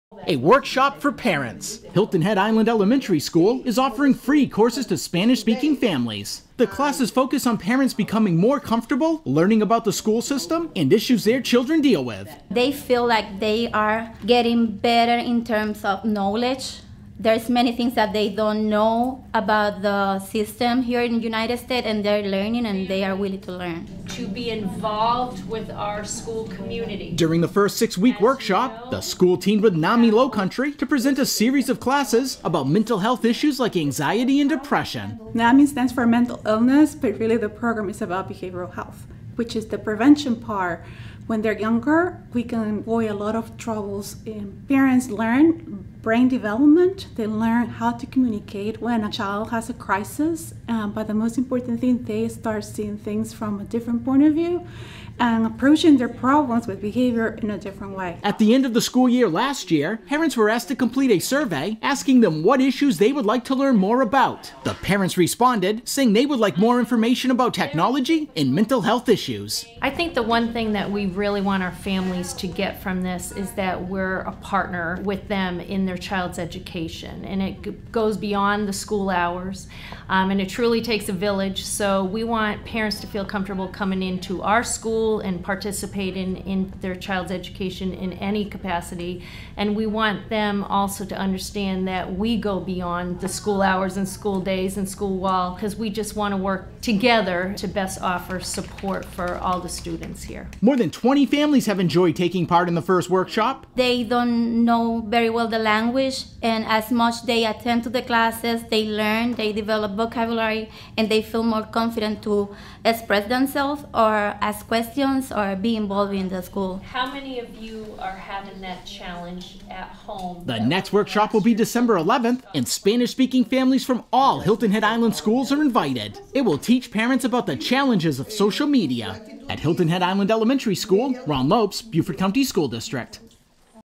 Parent Workshop
Hilton Head Island Elementary School teamed with NAMI Lowcountry to put on a series of parent education classes.